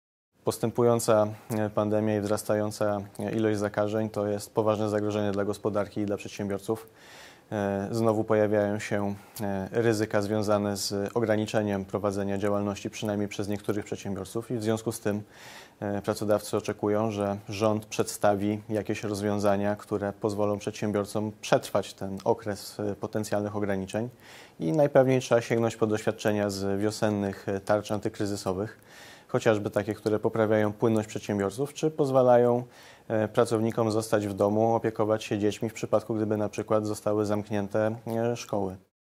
Piotr Wołejko o wsparciu dla przedsiębiorców w czasie pandemii
– W ramach dialogu z przedsiębiorcami i pracownikami można przygotować adekwatne rozwiązania, które wspomogą walkę z epidemią bez zamykania firm – mówi Piotr Wołejko, ekspert Pracodawców RP ds. społeczno-gospodarczych.